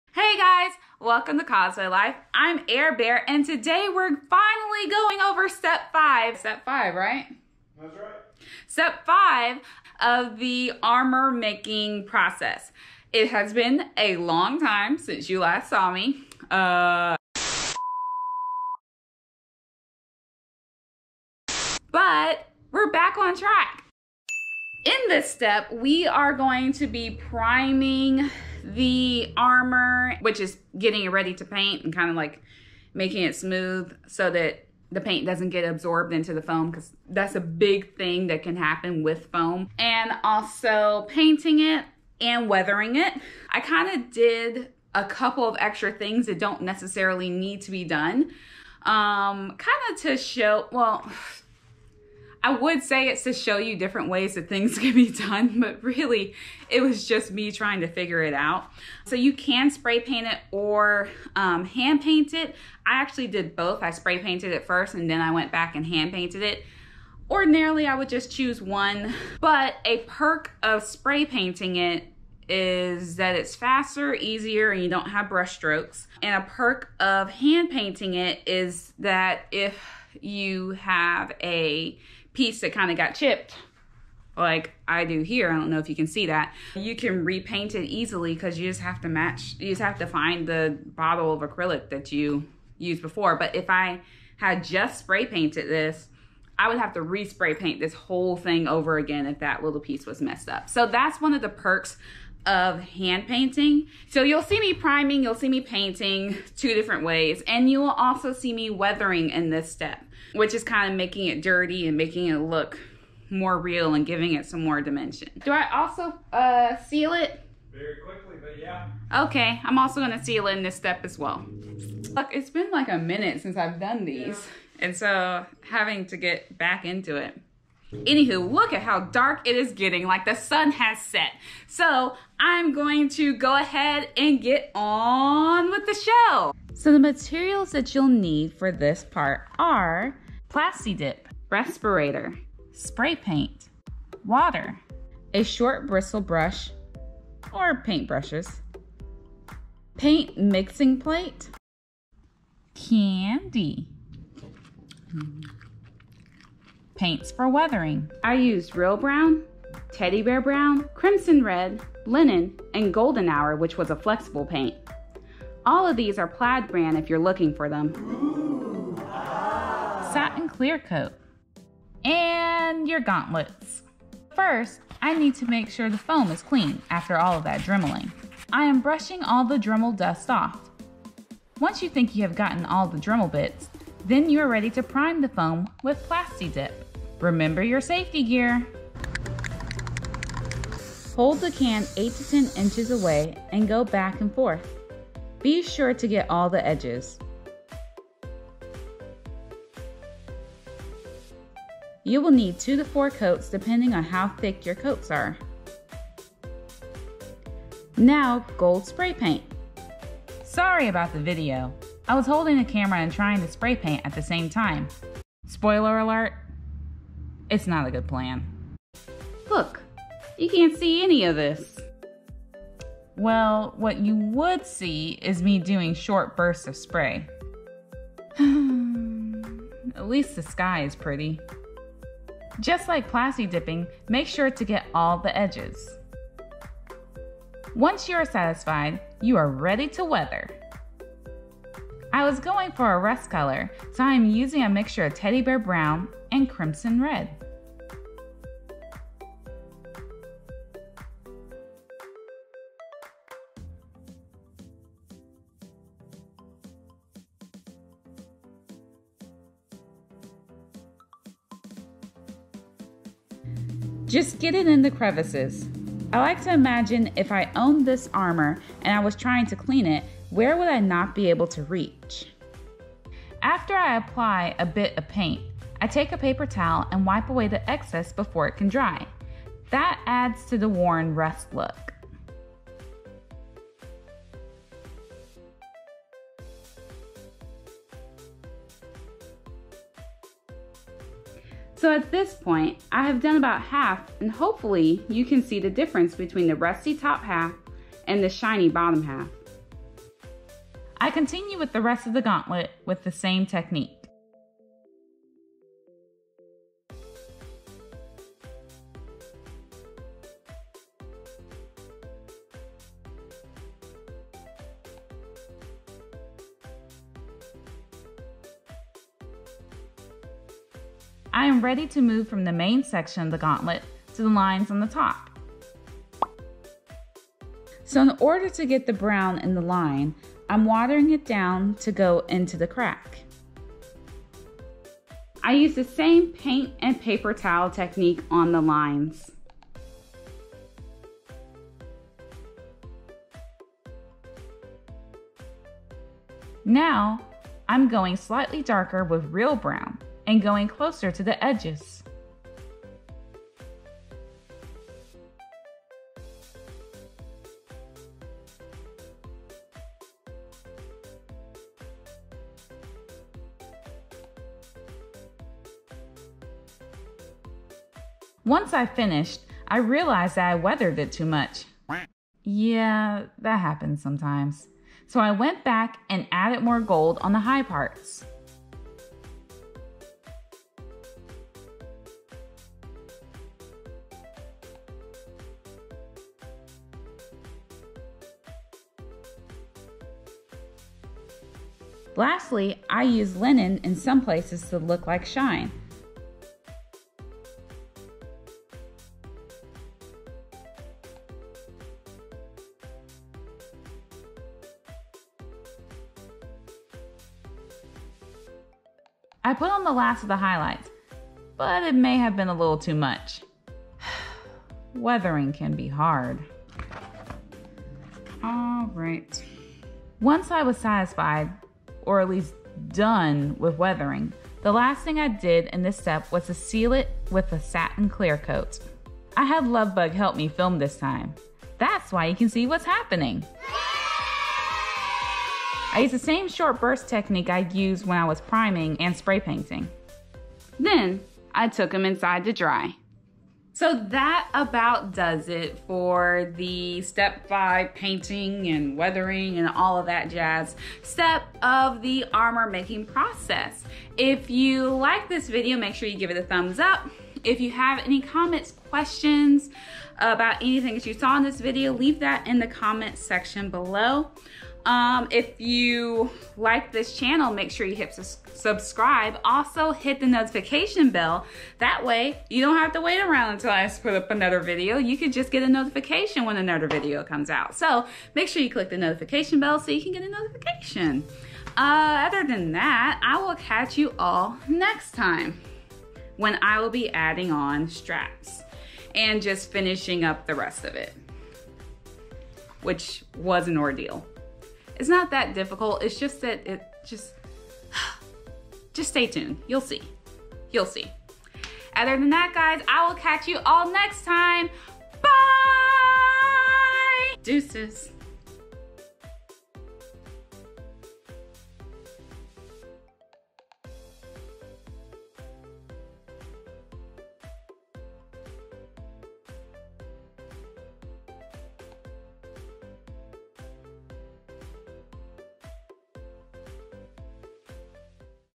Making Foam Armor - Part 5 Painting The Craft Foam
A tutorial for how to make cosplay foam armor (step 5). We continue the process with the 5th step which is painting and weathering the foam that we shaped in the last video.

Here are links to the videos in the series (we will update the list as they come out):